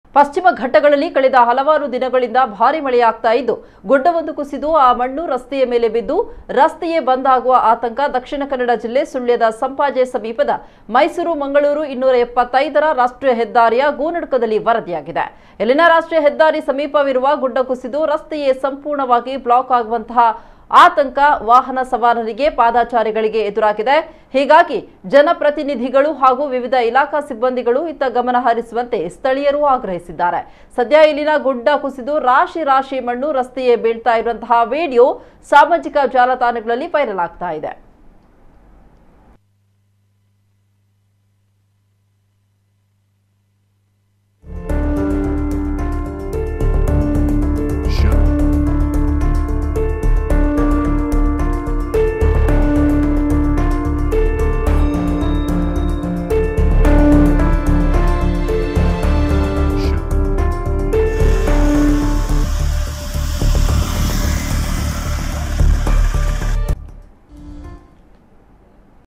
ಭಾರಿ ಮಳೆಗೆ ಸುಳ್ಯದ ಸಂಪಾಜೆ ಗೂನಡ್ಕದಲ್ಲಿ ಗುಡ್ಡ ಕುಸಿತ..!
ಪಶ್ಚಿಮ ಘಟ್ಟಗಳಲ್ಲಿ ಕಳೆದ ಹಲವಾರು ದಿನಗಳಿಂದ ಭಾರಿ ಮಳೆಯಾಗುತ್ತಿದ್ದು, ಗುಡ್ಡವೊಂದು ಕುಸಿದು ಆ ಮಣ್ಣು ರಸ್ತೆಯ ಮೇಲೆ ಬಿದ್ದು, ರಸ್ತೆಯೇ ಬಂದ್ ಆಗುವ ಆತಂಕ ದಕ್ಷಿಣ ಕನ್ನಡ ಜಿಲ್ಲೆ ಸುಳ್ಯದ ಸಂಪಾಜೆ ಸಮೀಪದ   ಮೈಸೂರು ಮಂಗಳೂರು 275ರ ರಾಷ್ಟ್ರೀಯ ಹೆದ್ದಾರಿಯ ಗೂನಡ್ಕದಲ್ಲಿ ವರದಿಯಾಗಿದೆ.